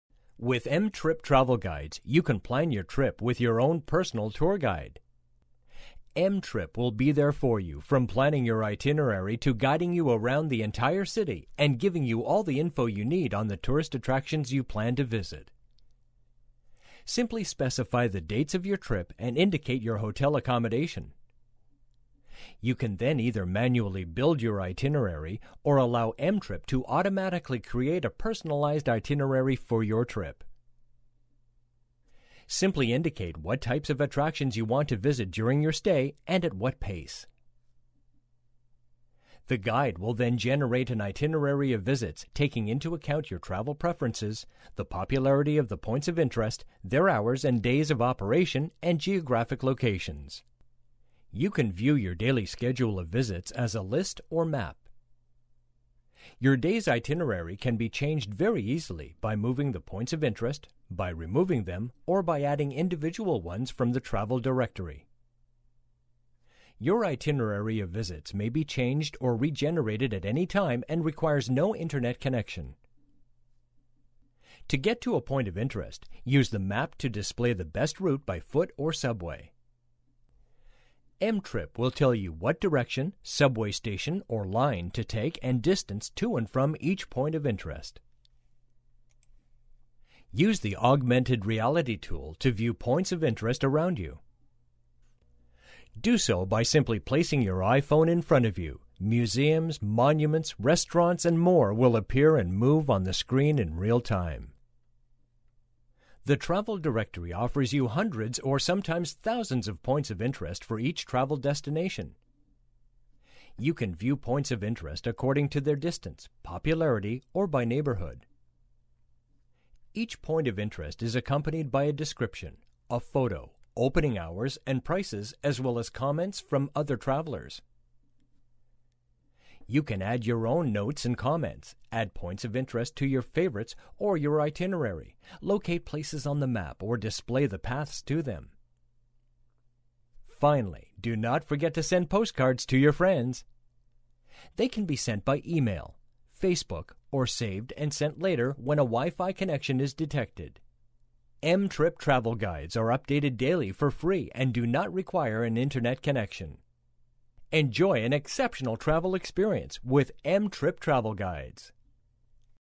mTrip Launches Travel Guides for iPhone and iPod Touch
mTrip Travel Guides for iPhone & iPod Touch hit the App store today with nine US and European city guides. Just like a personal tour guide, mTrip is there for you from planning your itinerary to guiding you around the entire city and describing tourist attractions, restaurants, hotels and more to you.